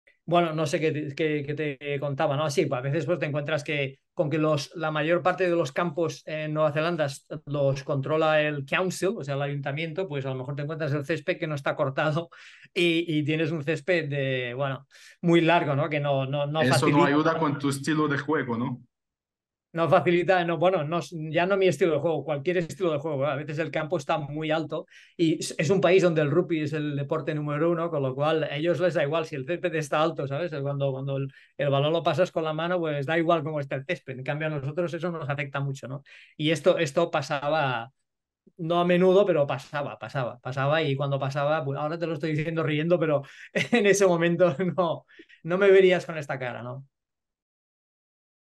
المدرب الكتالوني السابق لـ أوكلاند: واجهت صعوبة في نيوزيلندا بسبب طول عشب الملعب
رامون تريبوليتش المدير الفني التاريخي لـ أوكلاند سيتي يتحدث عن الصعوبات التي واجهها خلال سنواته في نيوزيلندا مع طول عشب الملعب.